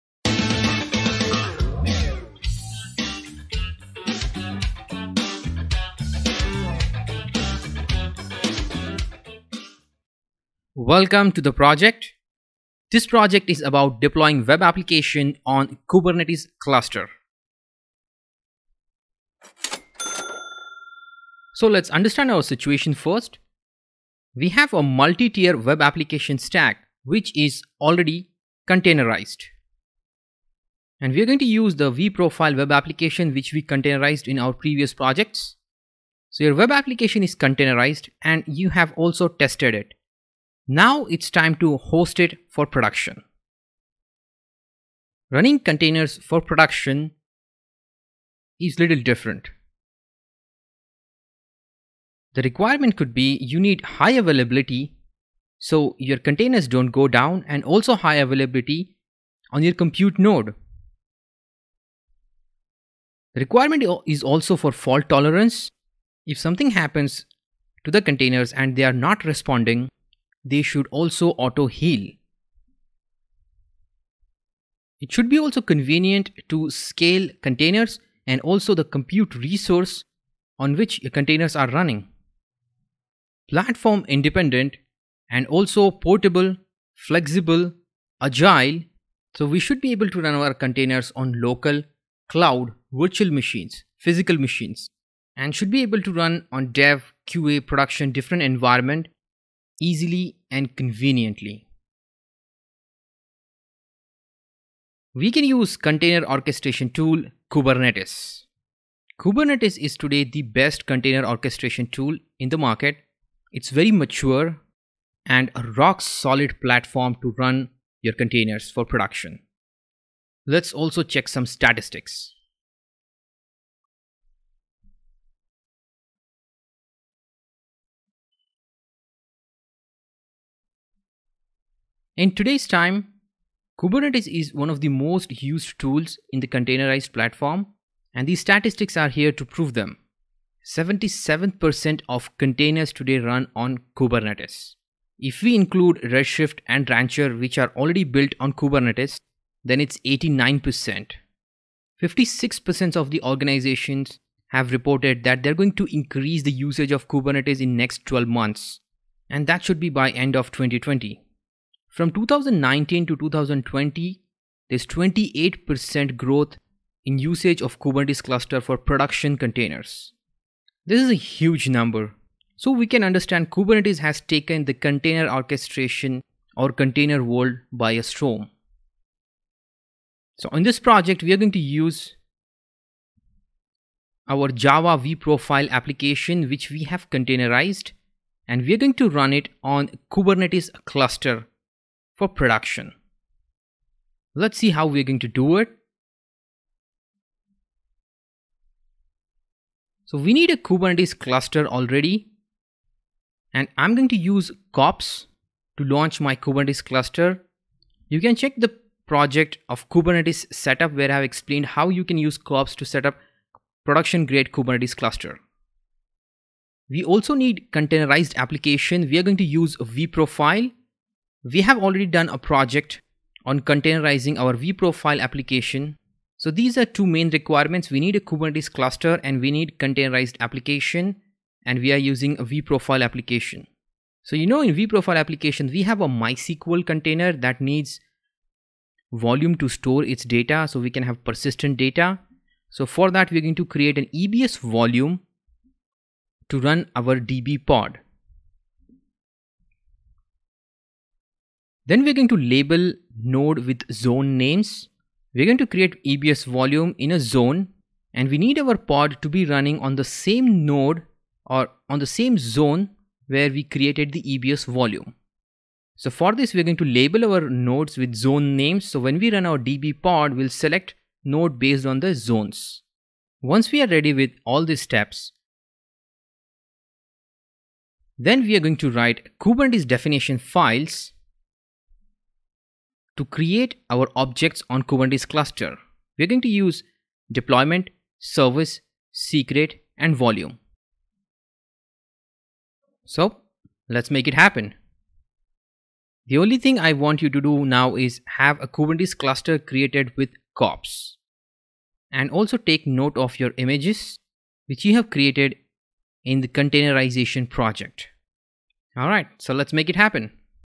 DevOps Real-time Project 16 || Java App Deployment on Kubernetes Cluster
In this project will discuss Introduction to Java App Deployment on Kubernetes Cluster.